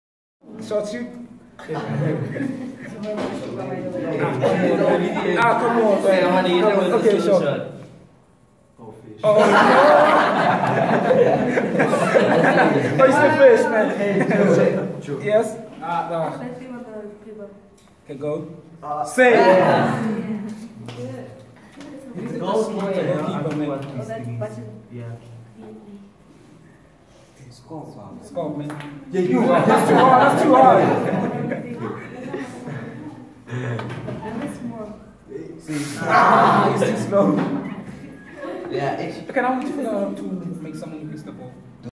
Techno Soccer Match
Getting creative with technology.  Workshops hosted by Dr. Michaella Janse van Vuuren of for the Deutsche Schule Pretoria Science week